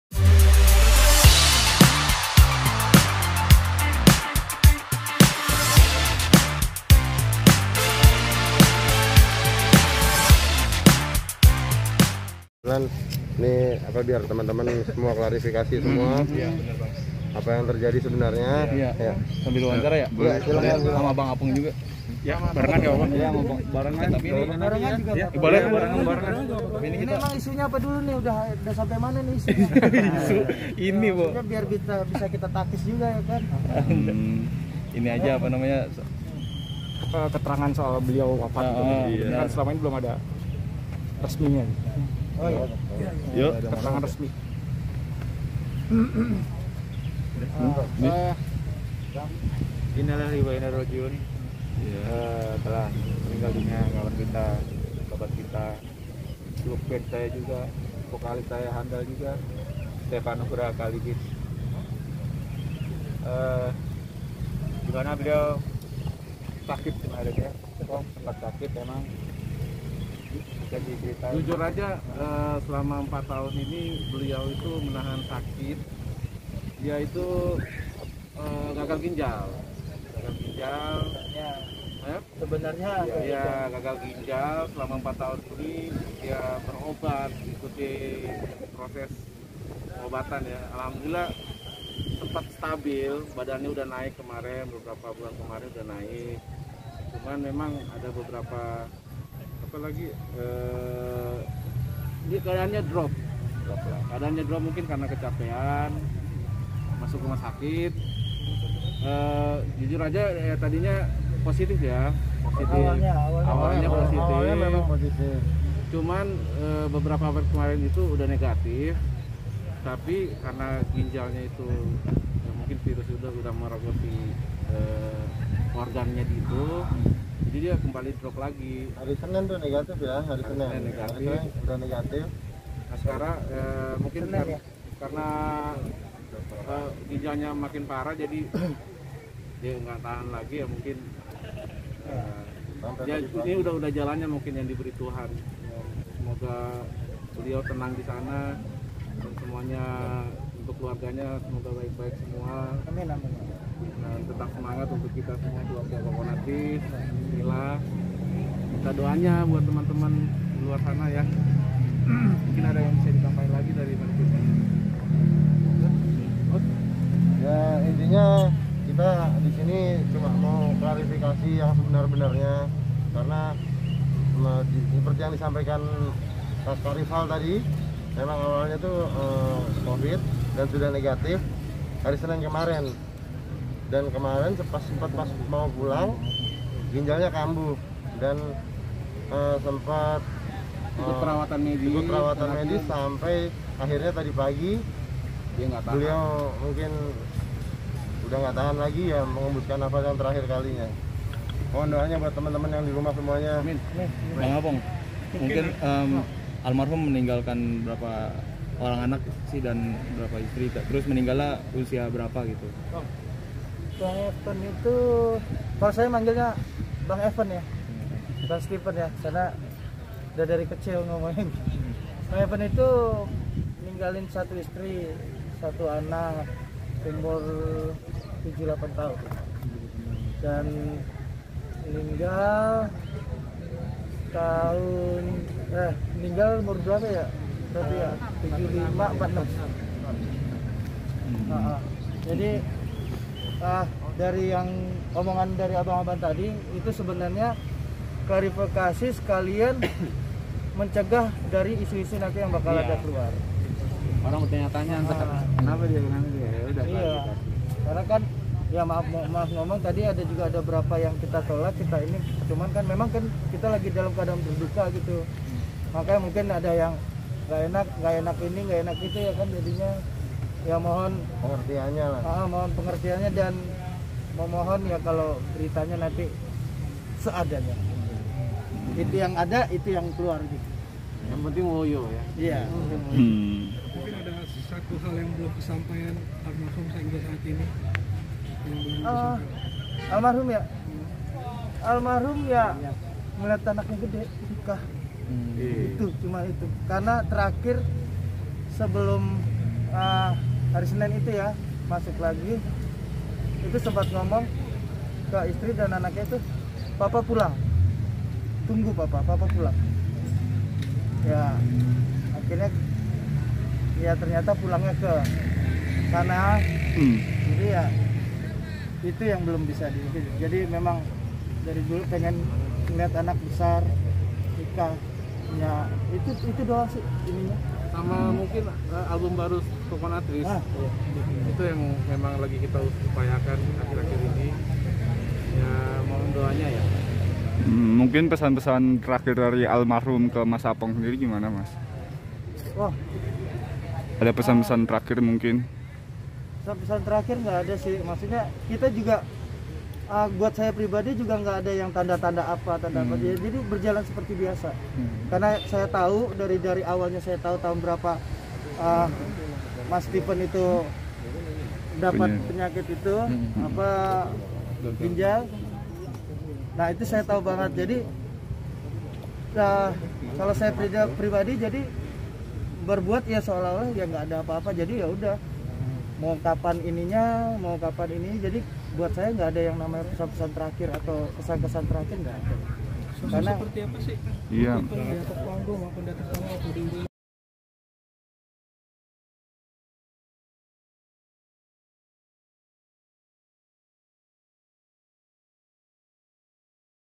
4 TAHUN TAHAN SAKIT, KELUARGA STEVEN AND COCONUT TREEZ UNGKAP KEBENARAN - UPDATE BINTANG
Steven N Kaligis, vokalis grup band Steven & Coconut Treez tutup usia saat ia sudah dinyatakan negatif pada Selasa (22/6) pukul 07.30 WIB. 4 Tahun Tahan Sakit, Keluarga Steven And Coconut Treez Ungkap Kebenaran.